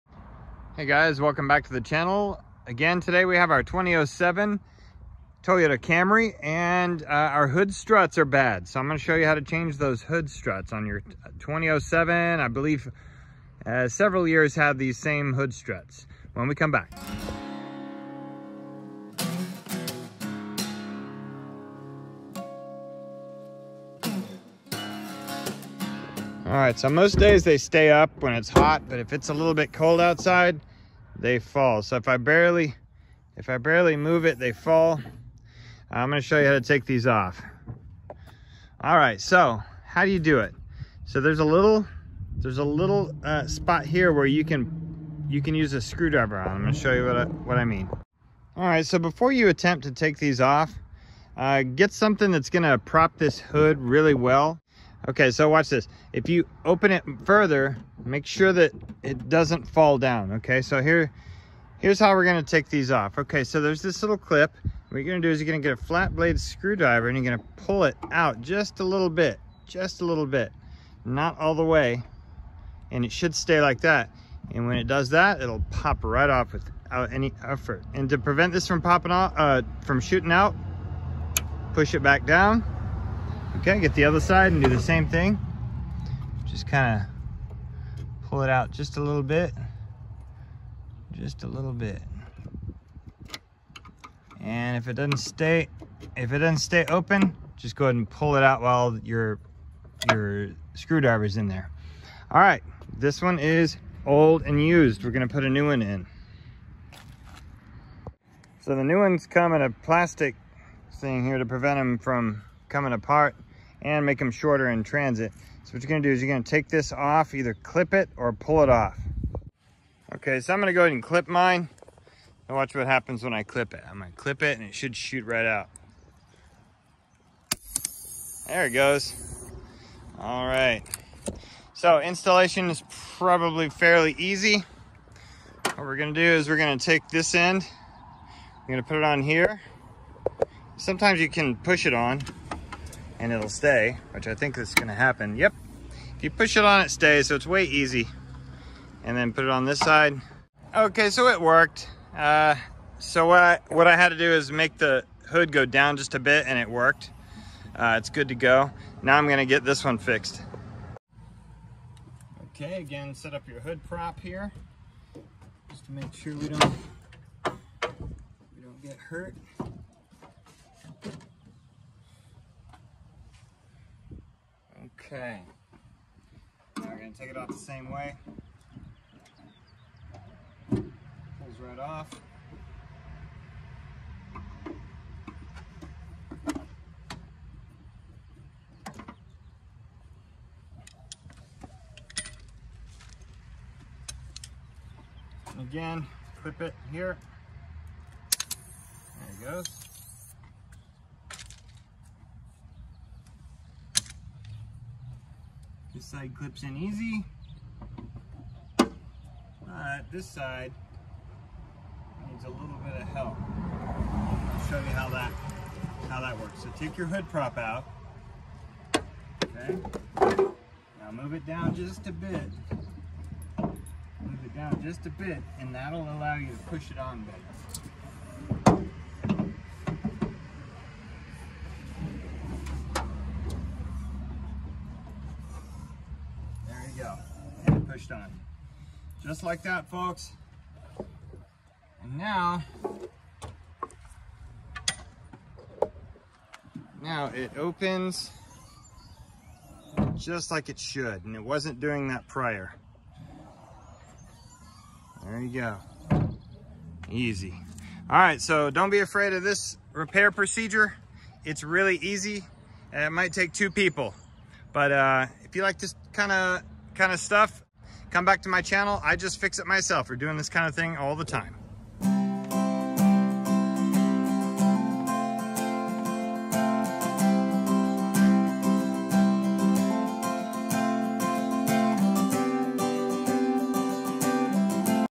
How to Replace Gas Hood Struts
These really are simple to change if you need them.  Most important step is to order the correct ones. Please observe safety when propping the hood of your car to prevent it from slamming shut!

This procedure is the same for most vehicles, but the car featured here is the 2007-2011 Toyota Camry.

Amazon link to the struts I ordered are here:

2 Pack Front Hood Lift Support Struts for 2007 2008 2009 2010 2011 Toyota Camry PM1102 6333